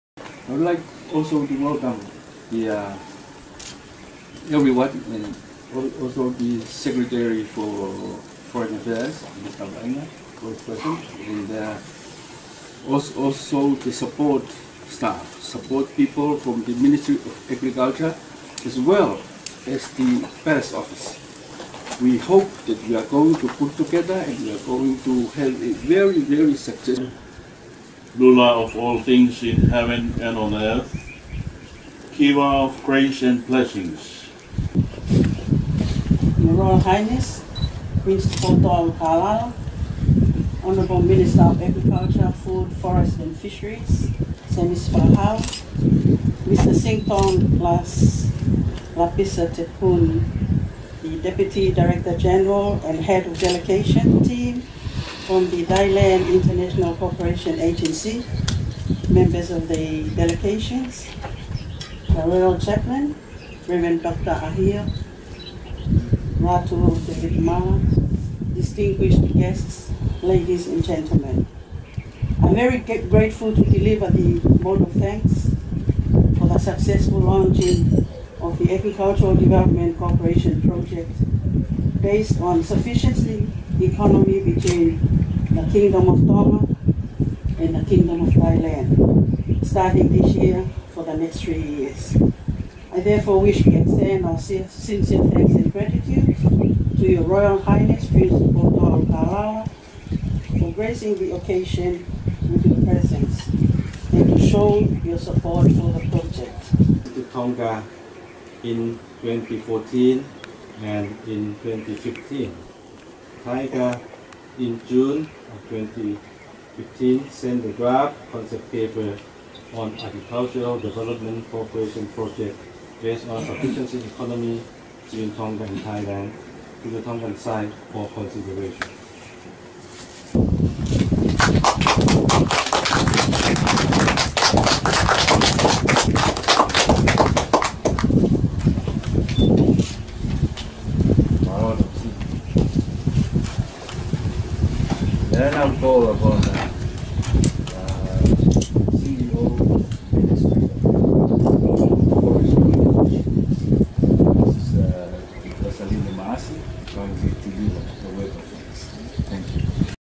Remarks launching Tonga project
Remarks by
Mr. Singtong Lapisatepun
Deputy Director – General of Thailand International Cooperation Agency (TICA), Ministry of Foreign Affairs
for the launching of the Tonga Thai Agricultural Development project
based on sufficiency economy
2 February 2016
Nuku’ alofa Tonga

HRH  Crown Prince Tupouto’a’ Ulukalala,
Your Excellency Mr. Semisi Fakahau, Minister of Agriculture, Food, Forestry and Fisheries,
Madam Losaline Ma’asi, CEO of Ministry of  Agriculture,
Mr. Jone Daulakeba,
Excellency,

On behalf of Thailand International Cooperation Agency (TICA), Ministry of Foreign Affairs of the Kingdom of Thailand, I would like to express my gratitude to Your Royal Highness, the Palace Office and the Ministry of Agriculture, Food, Forestry, Fisheries of Tonga for the warm welcome and hospitality extended to me and my colleague.    It is indeed my great honour to be in Nuku’alofa again and to be here for the launching of Agricultural Development project based on Sufficiency Economy between the Kingdom of Thailand and the Kingdom of Tonga.

Your Royal Highness,
Excellencies,

Please allow me to brief the overview of the project to Your Royal highness. May I refer to the meeting in Canberra in 2014 between the Director-General of TICA and the officials of accredited Embassy of the Kingdom of Tonga with the participation of Her Royal Highness Princess Angelika Latufuipeka Tuku’aho, the High Commissioner of the Kingdom of Tonga to the Commonwealth of Australia and the accredited Ambassador to the Kingdom of Thailand.  In the meeting, Tongan side expressed interest in cooperation between Thailand and Tonga in implementing the Agricultural Development Project based on Sufficiency Economy as the development model.

After two Thai missions visited Tonga in 2014 in 2015, TICA, in June 2015, sent the draft concept paper on “Agricultural Development Cooperation Project based on Sufficiency Economy between Tonga and Thailand”  to the Tongan side for consideration.  In October 2015, we received feedback from the Tongan side and agreed that the First Project Steering Committee meeting should be held early this year to discuss and finalize certain issues so that the concept paper can be agreed by both sides and the project activities identified in the work plan be started.

Your Royal Highness,
Excellencies,

 Under the project, there are four main activities to be implemented.  They are namely field activities, providing of equipment and materials, dispatching of short-term Thai experts and volunteers and technology transfer, including training of trainers.

 This morning we had the first Project Steering Committee meeting at the Ministry of Agriculture.  We had a fruitful discussion and agreed to the project management issues and I would like to ensure Your Royal highness that the project can be implemented soon in the first quarter of this year so that Tonga can have a demonstration plot for sustainable agricultural development based on sufficiency economy that Tongan people can learn from and apply in their farms.

 With this note,  again  on behalf of the Royal Thai Government, I would like to express my gratitude to Your Royal Highness and all Tonga agencies concerned for the warm welcome and the excellent hospitality extend to us, and reiterate our strong commitment to working closely with Tongan side to realize the project.